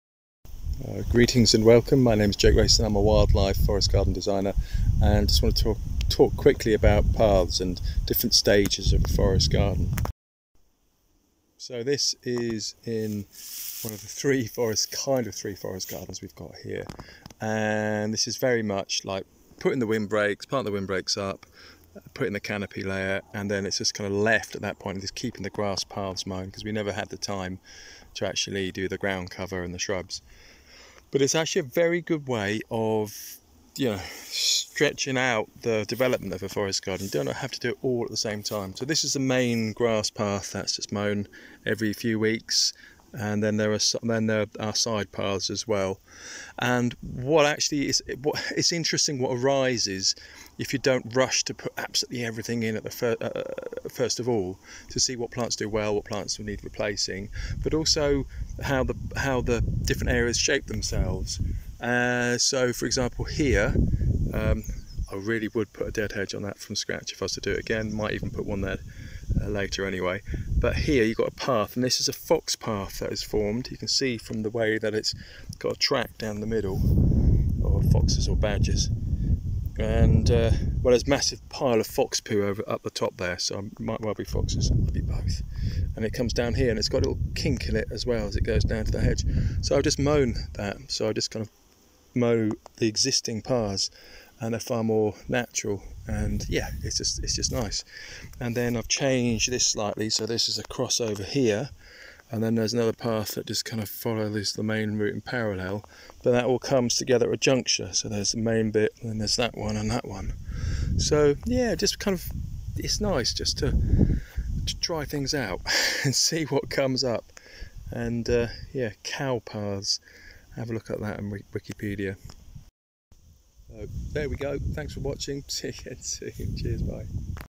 Where the wild things walk - paths in a forest garden
A quick look at the different stages of a ForestGarden -- remember, they will take *years* to grow, they are not an instant garden. This means you can grow the garden in stages

1. Infrastructure - paths, services
2. Canopy - windbreaks, dead hedges & trees
3. Shrubs & ground cover - area by area, no need to do all at the same time

Also, work with existing topography, paths formed where the wild animals walk